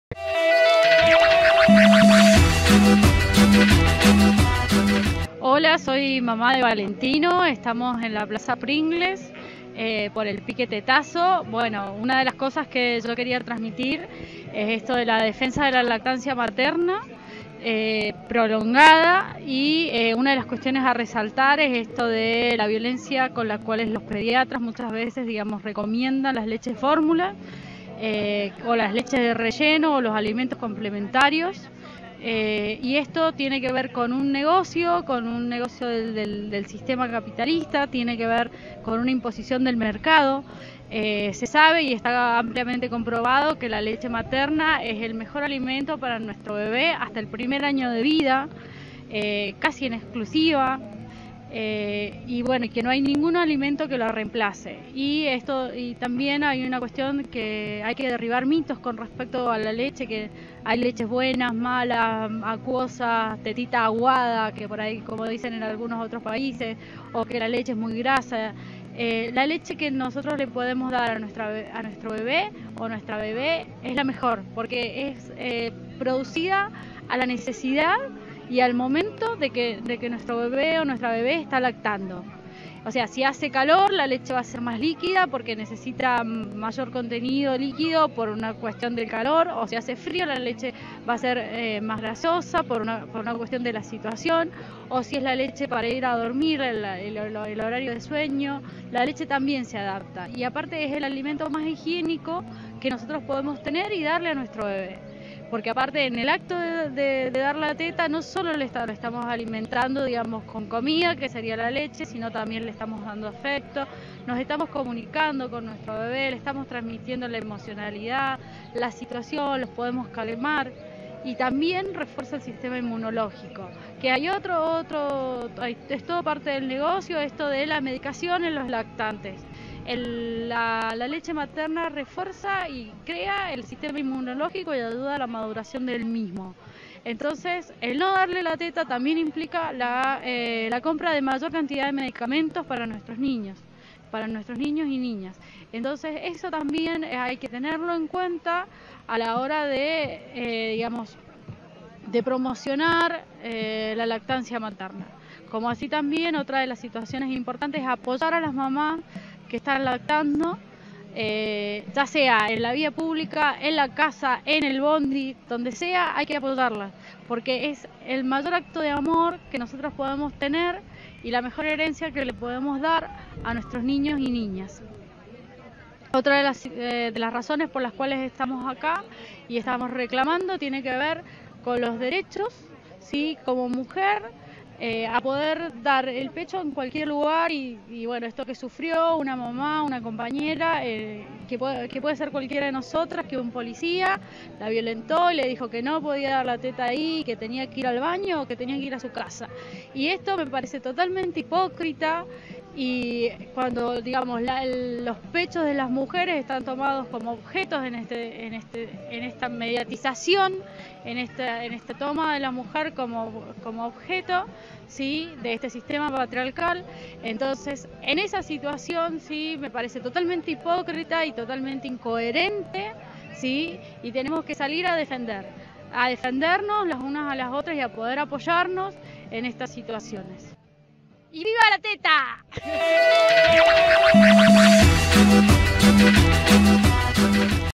Piquetetazo en San Luis, Betina
Volvimos a las calles, a las plazas , esta vez a decirle a la sociedad heteropatriarcal capitalista que la teta de una mujer que amamanta no debe ser tapada. No debe ni puede ser escondida bajo la doble moral que nos quieren imponer lxs mismxs que nos venden tetas cosificadas.

Salimos a las plazas de todo el país, a decirle ¡BASTA! a quienes que se horrorizan frente a una madre que muestra su teta porque está amamantando, pero que todas las noches se inmoviliza horas frente a la pantalla de TV viendo a mujeres de plástico bailando cuasi desnudas y tratadas como tontas descerebradas.

Salimos a las calles de todo el país a grítale ¡MUESTRO LO QUE QUIERO! a aquellxs que se avergüenzan de una teta amamantando en la vía pública, pero ni se percatan de las gigantografías publicitarias que exhiben mujeres con todo su cuerpo estereotipado como la más bella conejita de Play Boy.

Salimos, y seguiremos saliendo a las calles, a las plazas, a MOSTRAR CON ORGULLO NUESTRAS TETAS QUE AMAMANTAN, porque sabemos que es una derecho protegido por ley, que puede ser llevado a cabo donde la madre desee y considere necesario.

Salimos a las plazas, porque al igual que Coni Santos, la mamá increpada por ineptos policías porteños impidiéndole amamantar en la vía pública, muchas pasamos por situaciones similares, por miradas prejuiciosas o por comentarios seudo moralistas frente al simple y sagrado hecho de amamantar. Porque, como ya dijimos, la teta orgullosa que amamanta, a muchxs, asusta.

Salimos y seguiremos saliendo a las calles, a las plazas, porque las mujeres sabemos que la fuerza que empodera es de las que “tienen sangre en las venas, leche en las tetas y amor en el corazón”.